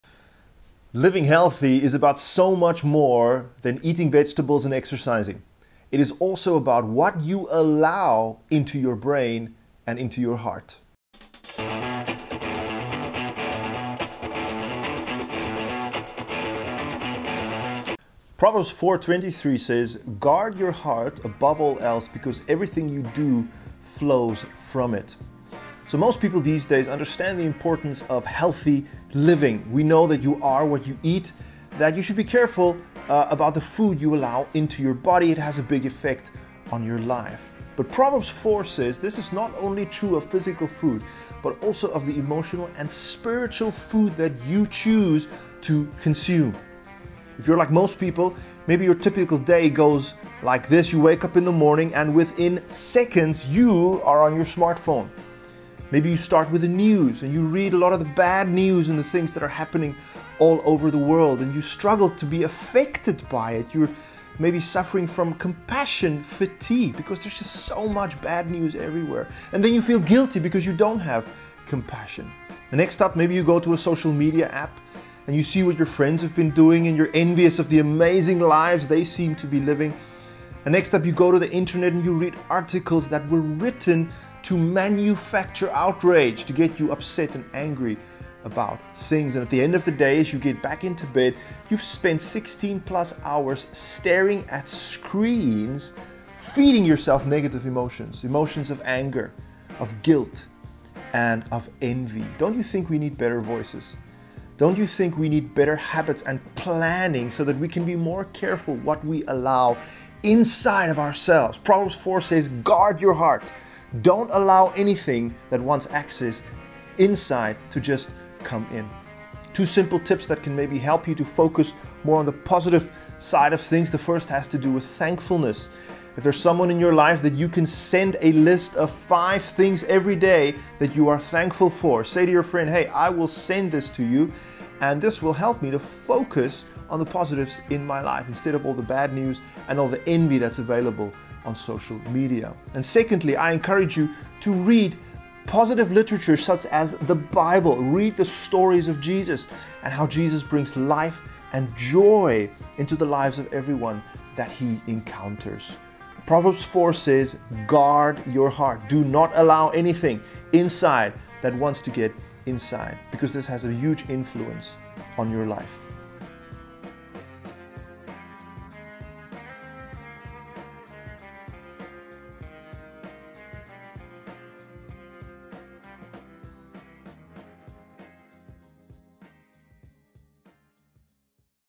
Was lässt du in dein Herz?
Gesund zu leben bedeutet so viel mehr als Gemüse zu essen und Sport zu treiben. Es geht auch darum, was du in dein Gehirn und in dein Herz lässt. Was bedeutet es, dein Herz zu "bewachen"?